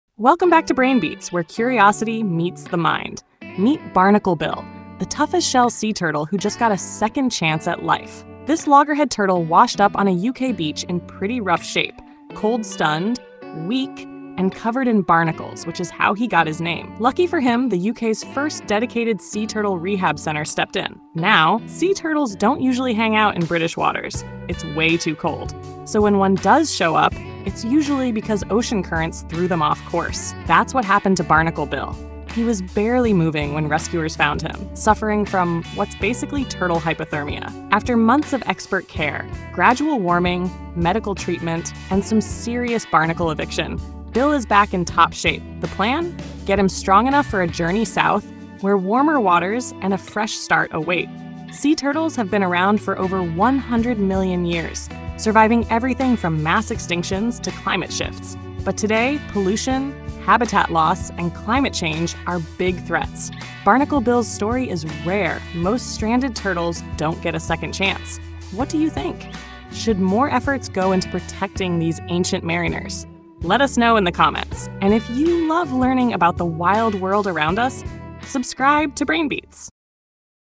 Barnacle Bill: The Resilient Sea Turtle
Meet Barnacle Bill, the tough little sea turtle who survived against all odds! Rescued in the UK, he was covered in barnacles and struggling to survive. Thanks to dedicated rehabilitation efforts, he’s getting a second chance at life. But why do turtles get covered in barnacles, and how does rehabilitation work? Dive in to find out!

Source:
Sky News - Meet Barnacle BillBarnacle Bill